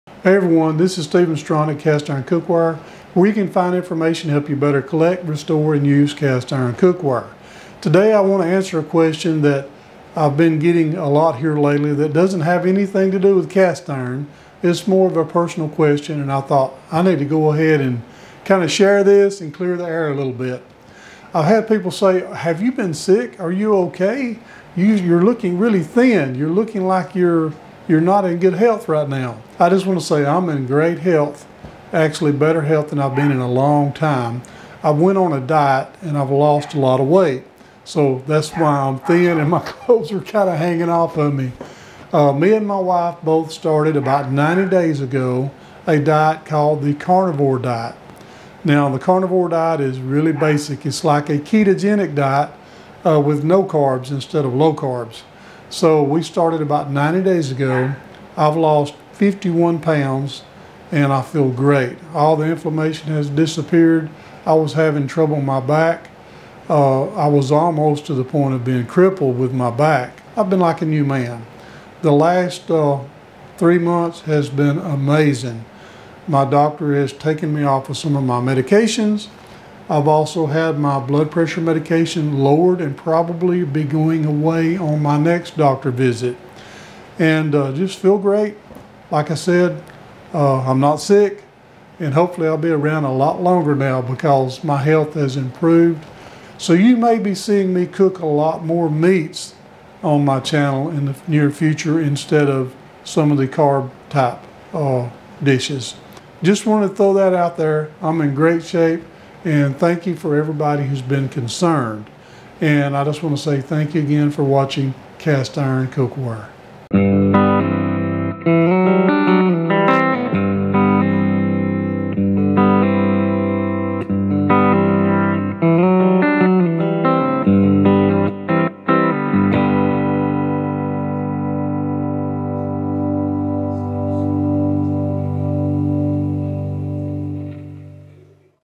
My Health Update
My Health Update is a short video where I give an update on my health because of the many questions that I have received. I have been on the Carnivore diet for 90 days and have lost over 50lbs. I feel great and my overall health has improved dramatically.

Home of "Easy Beezy" Cast Iron Seasoning Stick!

Recommended by Cast Iron Cookware

Lodge L9OG3 Cast Iron Round Griddle, Pre-Seasoned, 10.5-inch